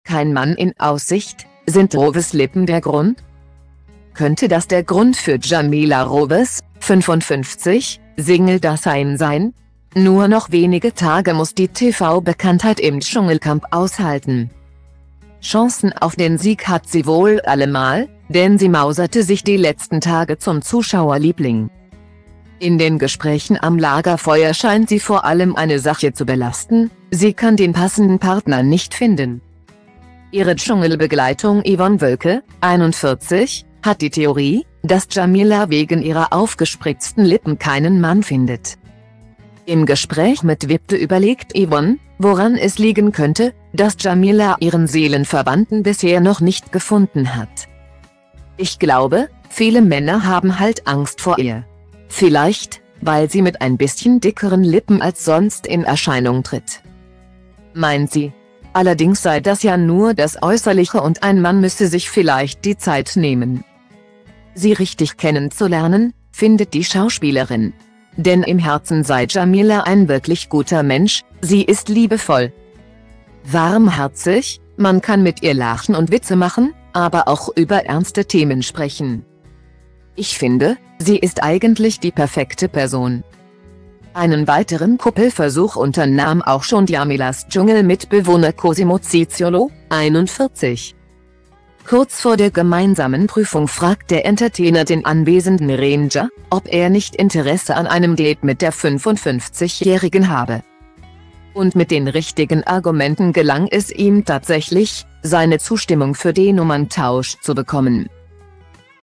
Kein Mann in Aussicht: Sind Rowes Lippen der Grund?
Könnte das der Grund für Djamila Rowes (55) Single-Dasein sein? Nur noch wenige Tage muss die TV-Bekanntheit im Dschungelcamp aushalten.Chancen auf den Sieg hat sie wohl allemal, denn sie mauserte sich die letzten Tage zum Zuschauerliebling.In den Gesprächen am Lagerfeuer scheint sie vor allem eine Sache zu belasten: Sie kann den passenden Partner nicht finden.